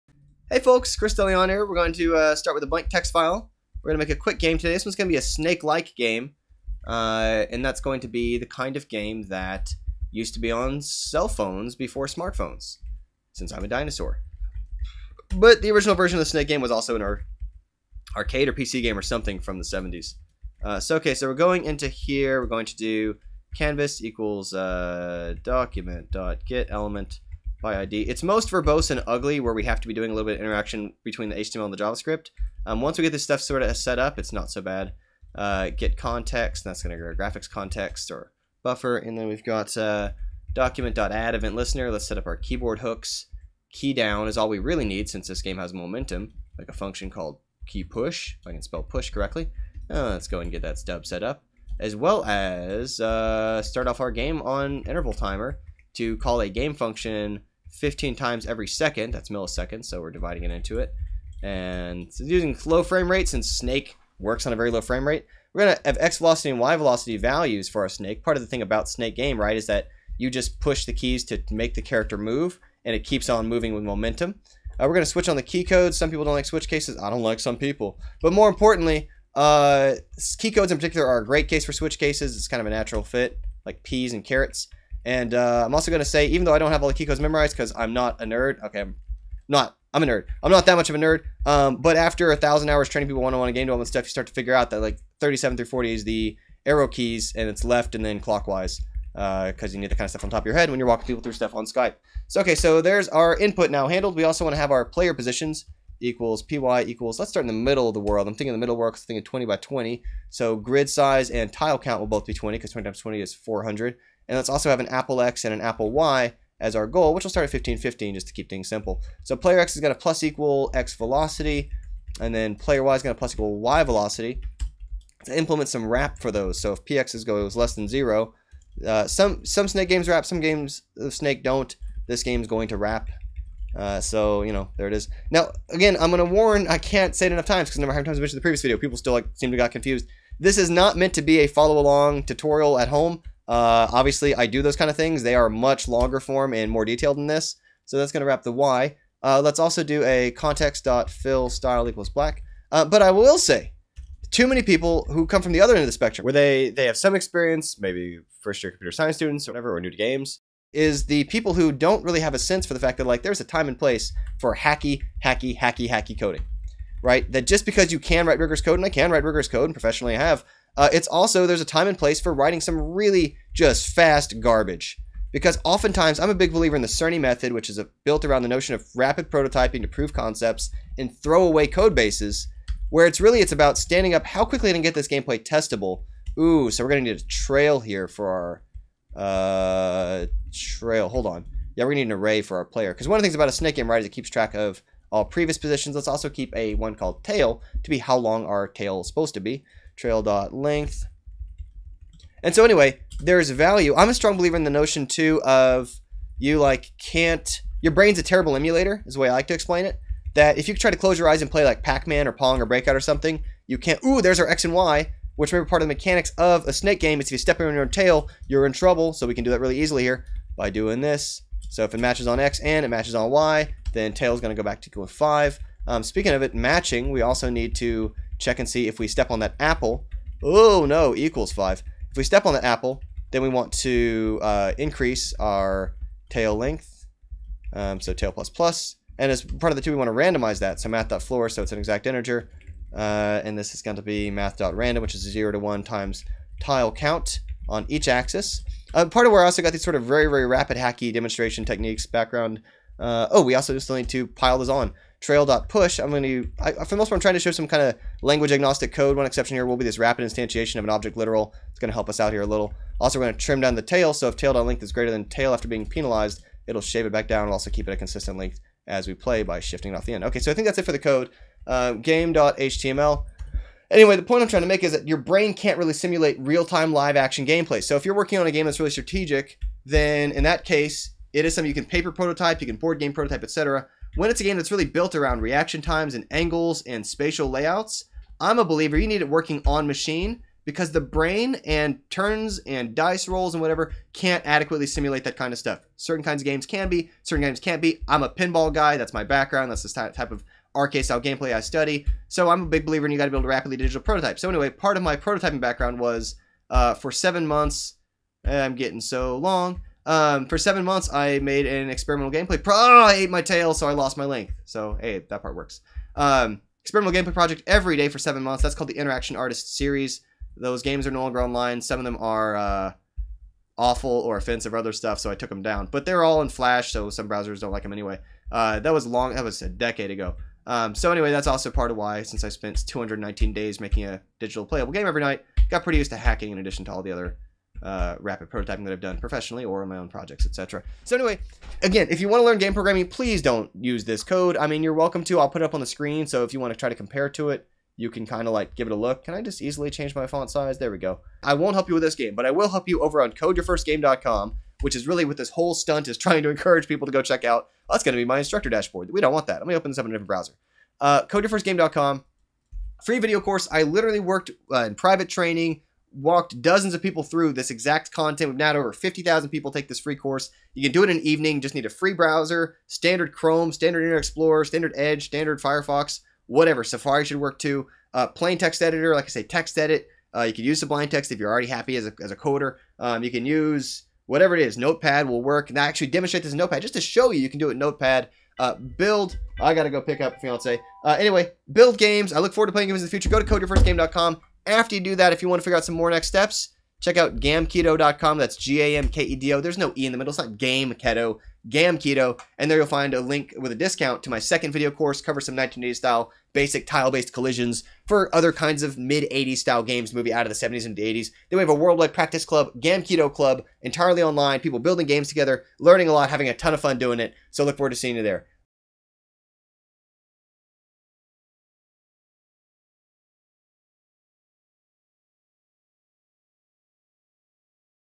Coding "Snake" in 4 min 30 sec (plain browser JavaScript)
(previously called Gamkedo Club) Free weekly tips in your inbox Free intro course The (hacky) Snake source About InteractionArtist, my 219 daily prototypes